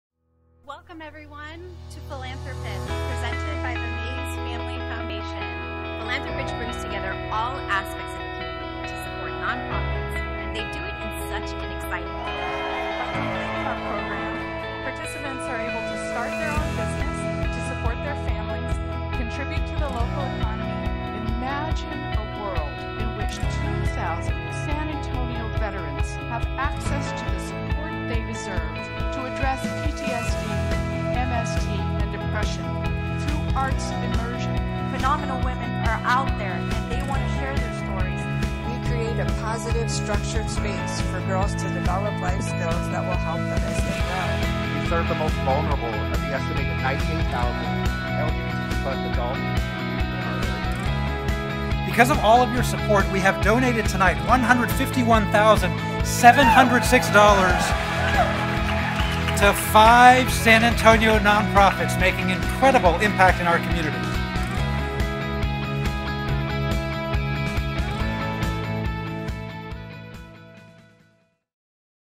Philanthropitch San Antonio 2021
Revisit the excitement of donating over $150,000 to five amazing San Antonio Nonprofits. Congratulations to Culturingua, Maestro Entrepreneur Center, Bihl Haus Arts, Pride Center San Antonio, and Girls on the Run.